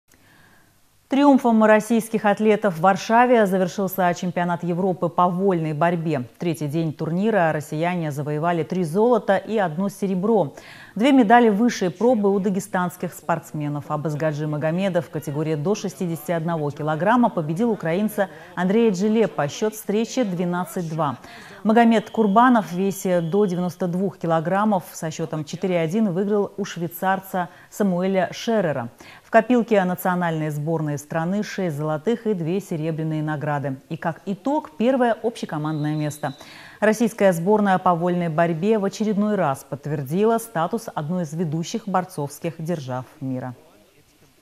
Сборная России заняла первое место на ЧЕ по вольной борьбе
Триумфом российских атлетов в Варшаве завершился чемпионат Европы по вольной борьбе. В третий день турнира россияне завоевали три золота и одно серебро. Две медали высшей пробы у дагестанских спортсменов.